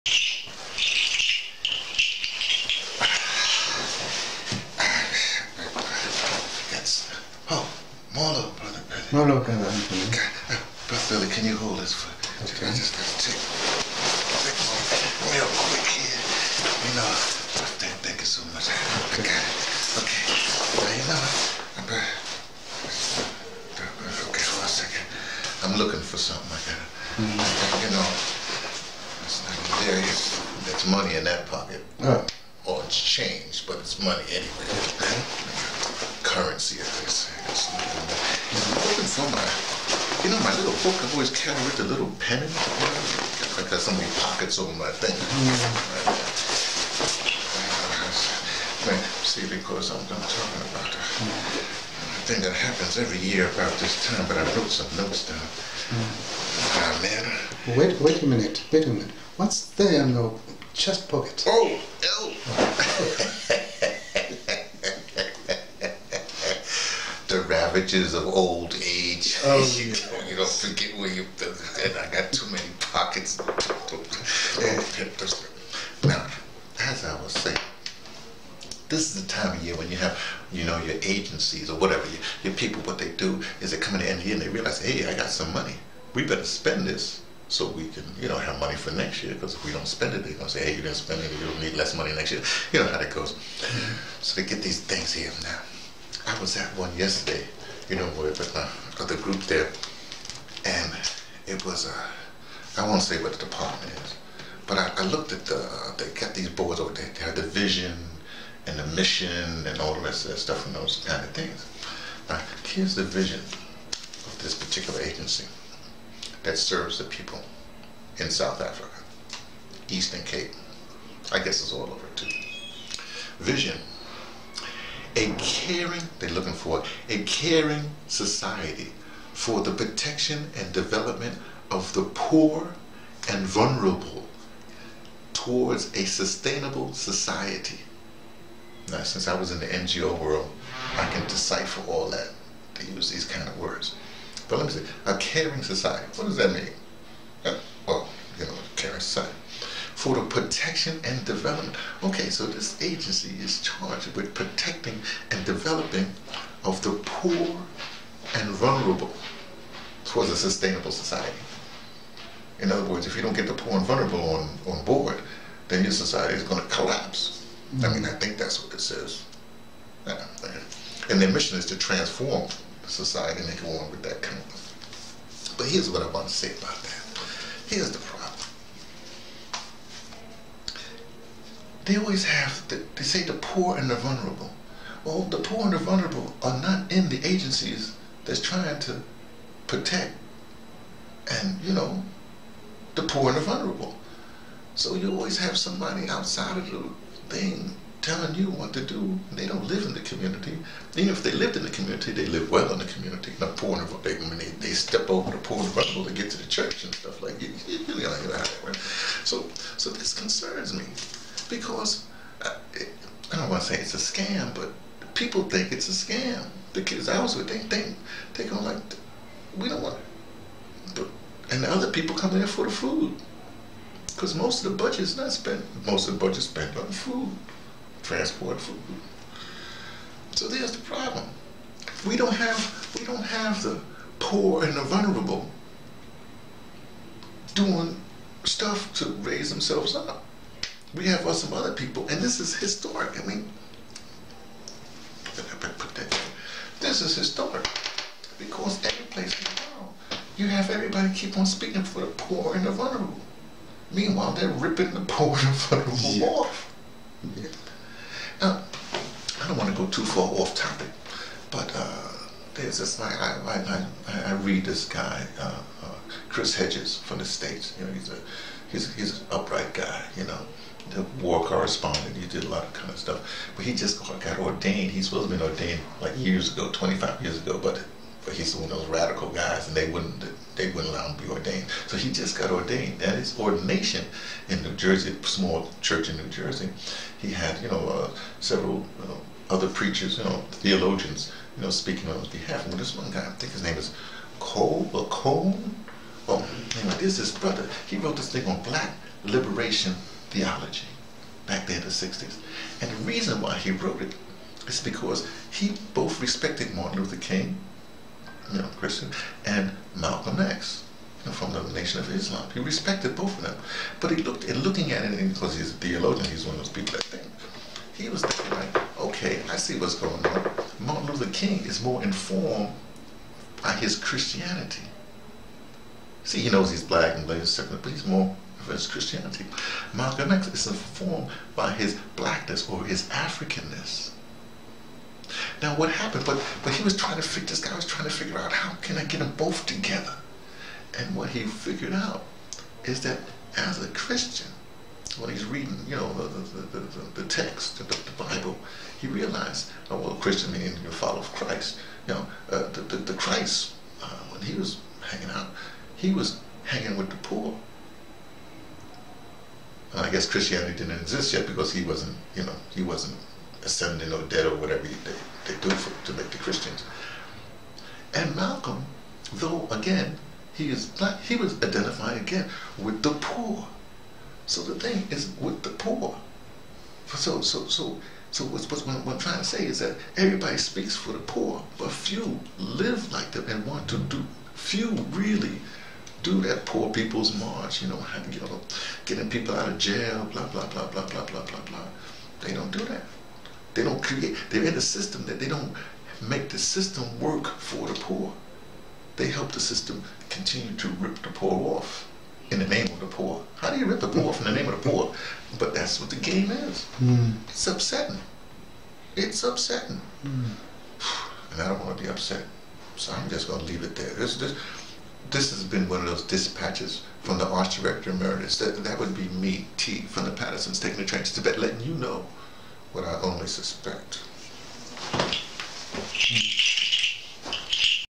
Exploiting the Powerless_mpeg4
This is a Dispatch from the ArtsDirector emeritus - (nom de você) - T (from thePatterson's takingthetraintoTibet) Tirade. In this Episode theBrother is talking about the political wealth gap and preying on the hapless.

(This Dispatch is a repose from a posting of October, 2014.)